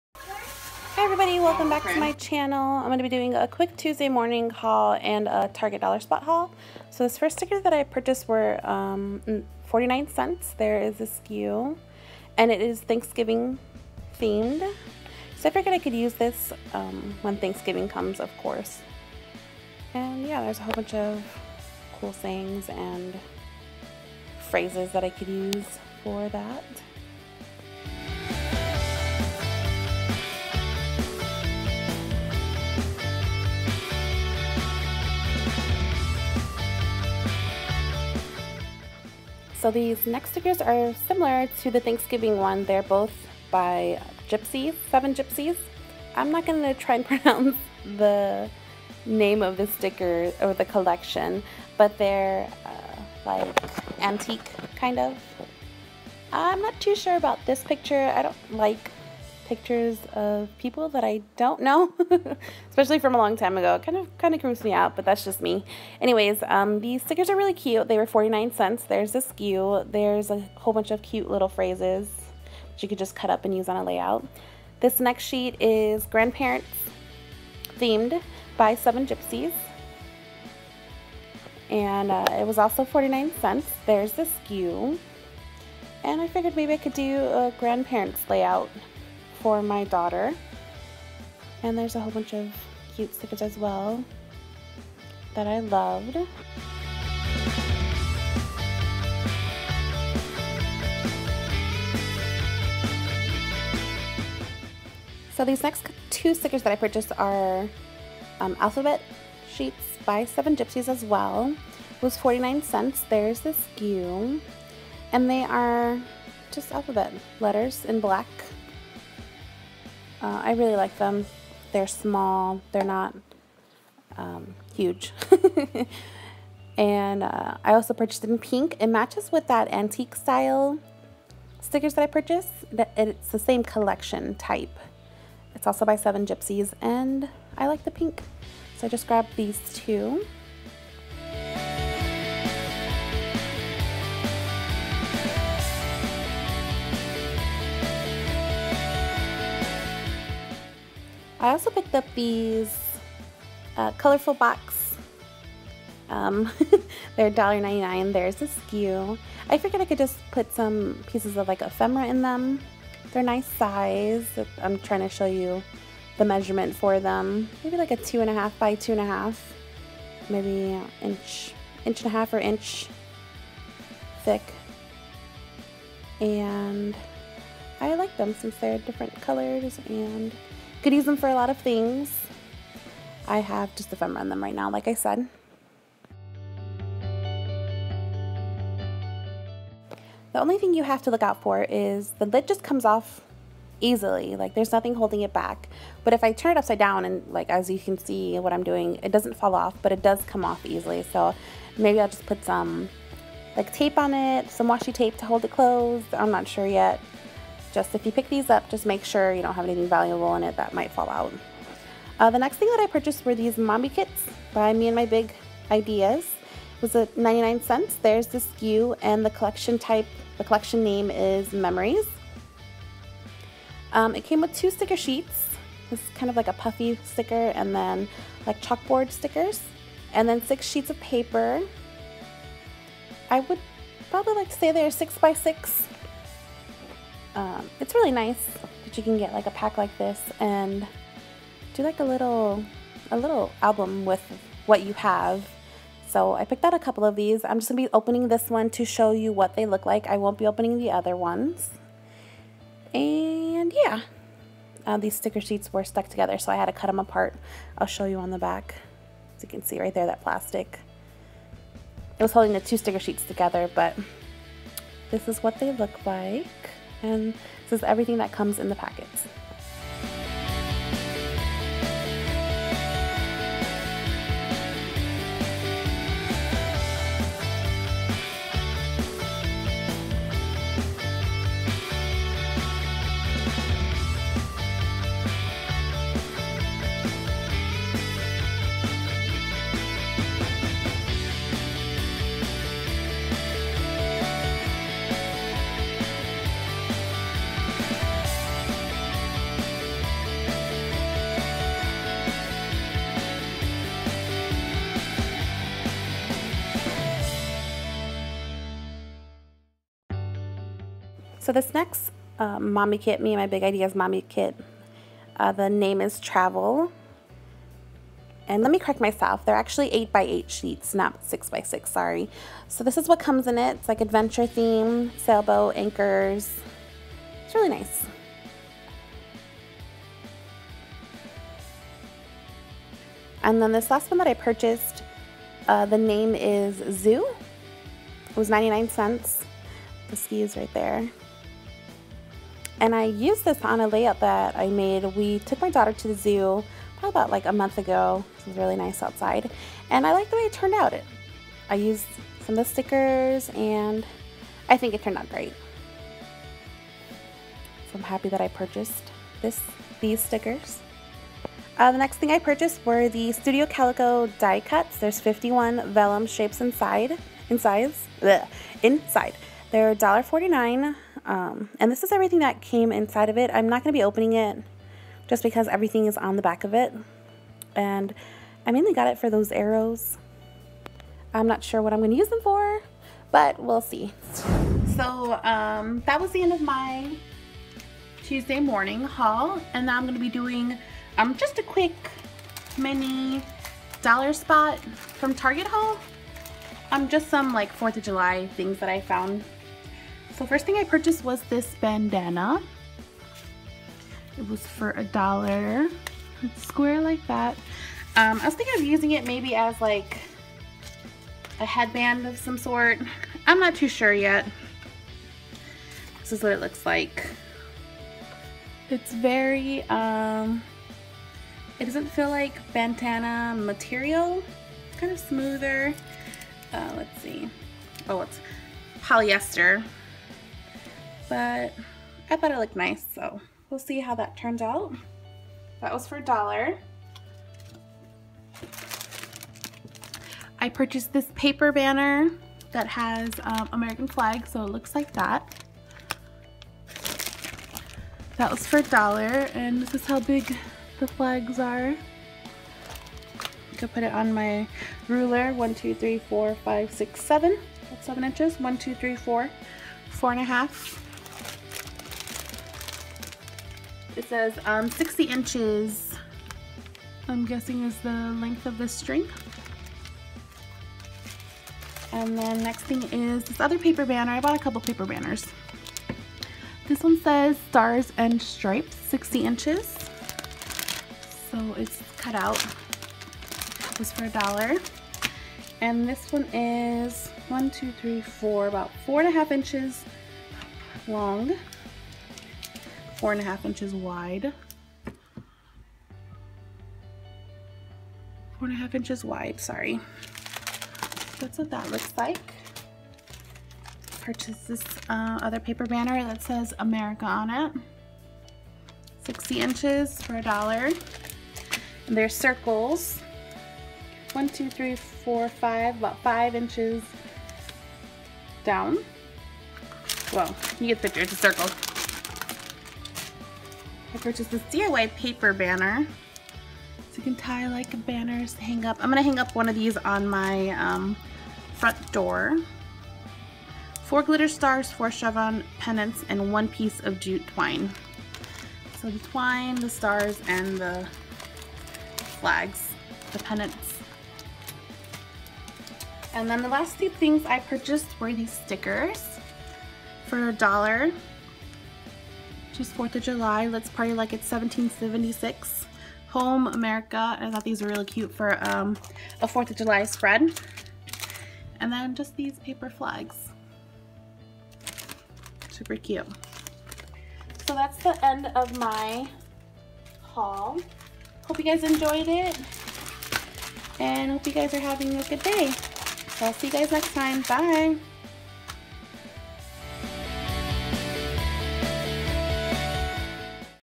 Tuesday Morning & Target dollar spot Haul!
I went to Tuesday morning and target on two different days ! I figured I could put them both in 1 video ! If you have any questions please leave them below !